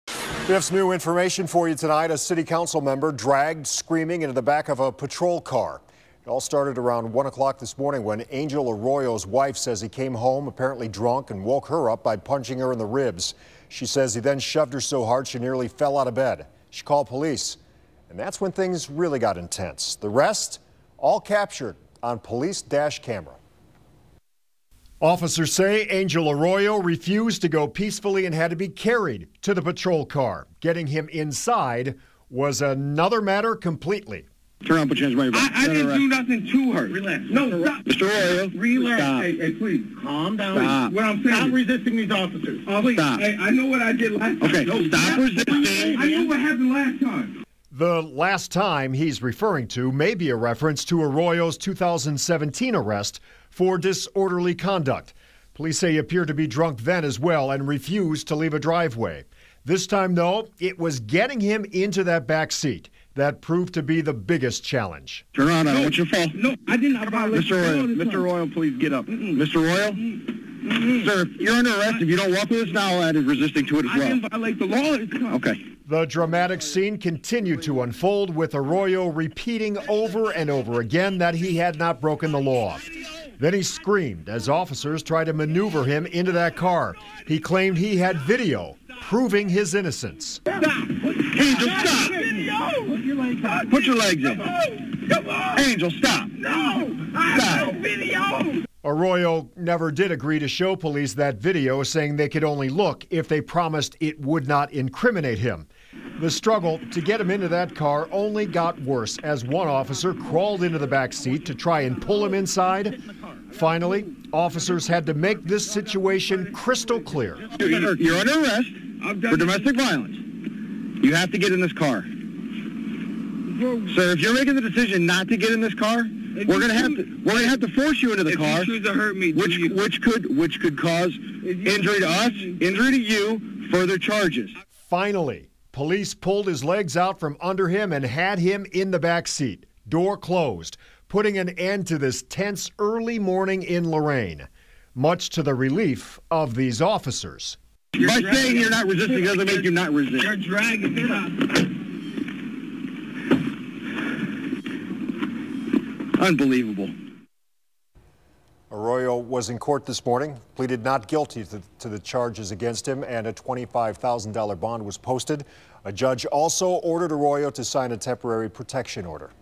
Lorain councilman arrested after domestic violence incident at his home, police say
Lorain councilman arrested on domestic violence charge